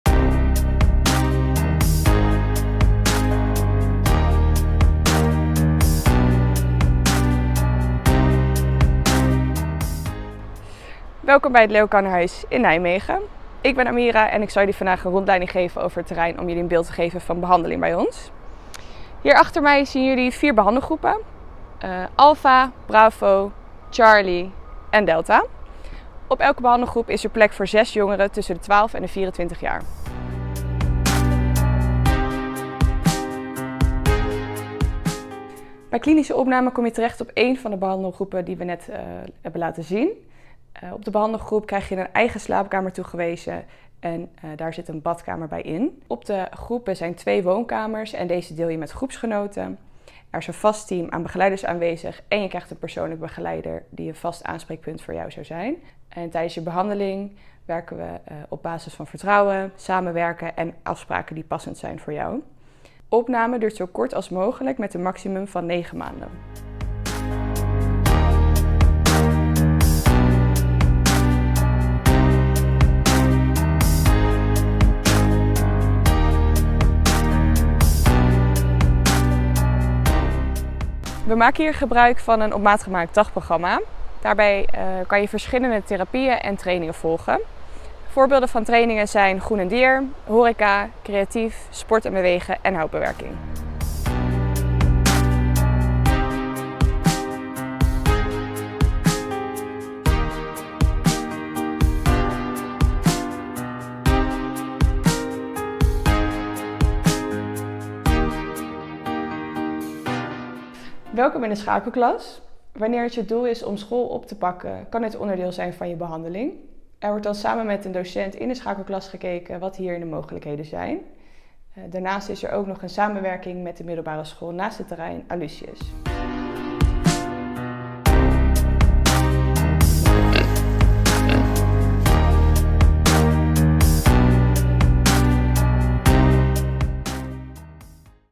Kennismaking met het Behandelcentrum Jongeren van Youz - Leo Kannerhuis in Nijmegen
Maak kennis met ons Behandelcentrum Jongeren in Nijmegen: een plek voor jongeren met autisme én andere psychiatrische en gedragsproblemen!

Bij het Leo Kannerhuis Behandelcentrum Jongeren behandelen we jongeren die vastlopen in hun ontwikkeling en te kampen hebben met uitdagingen op school, thuis of in hun sociale omgeving. Voor deze jongeren, waarbij eerdere behandelingen niet hebben geholpen, bieden wij een speciale plek om te werken aan hun problemen.

- Voor jongeren tussen 12 en 24 jaar.
- Tijdelijke opname met een duur van maximaal 9 maanden. Onze behandeling is volledig op maat gemaakt, waarbij we rekening houden met de individuele behoeften van elke jongere.
- Een gevarieerd en op maat gemaakt dagprogramma, met o.a. trainingen en therapieën op het gebied van groen en dier, horeca, creatief, sport en beweging en houtbewerking.
- We hebben een schakelklas met als doel om school weer op te pakken. Ook hebben we een samenwerking met de middelbare school Aloysius.

Wil je meer weten over ons behandelcentrum en wat we kunnen bieden? Neem gerust contact met ons op. Meer informatie?